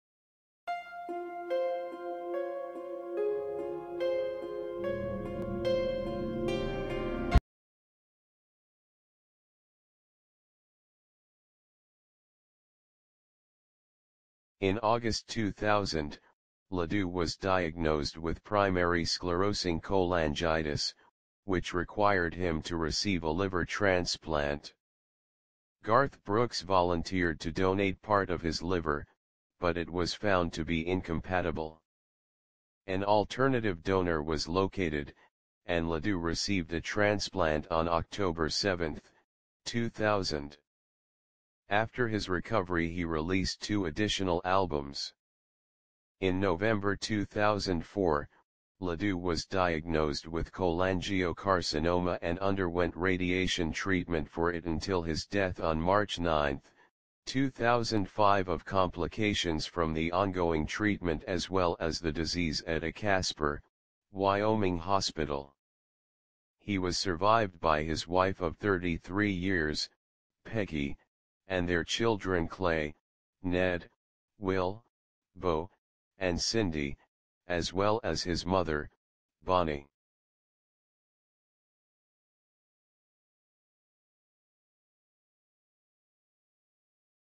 THE DEATH OF CHRIS LEDOUX
This video details the death of country singer Chris Ledoux!